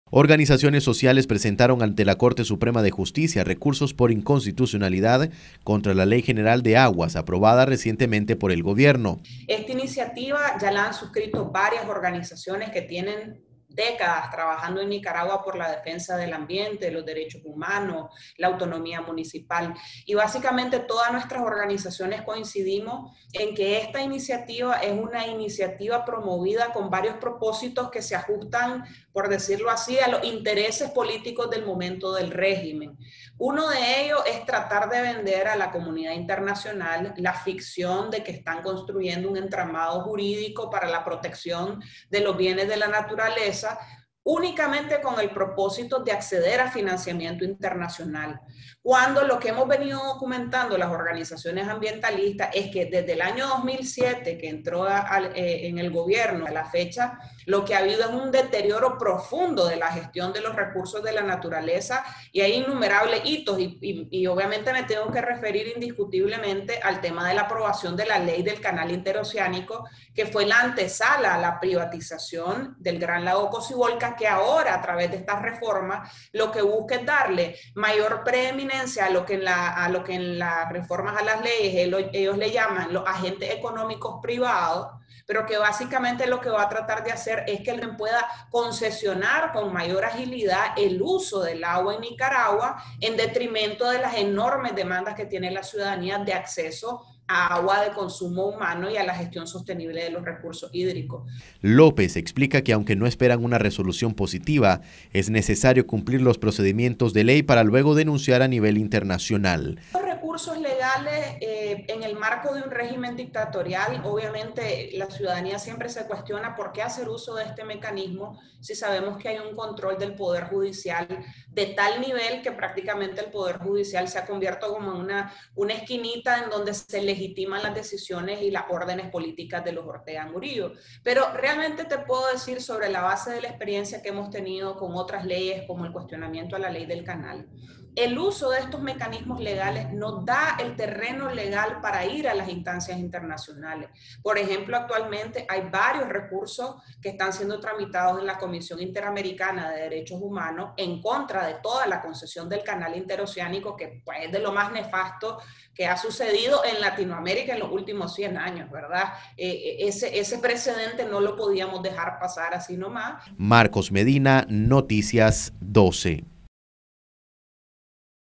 Recurren por inconstitucionalidad contra ley general de aguas
NosResistimosAlEmbargo Recurren por inconstitucionalidad contra ley general de aguas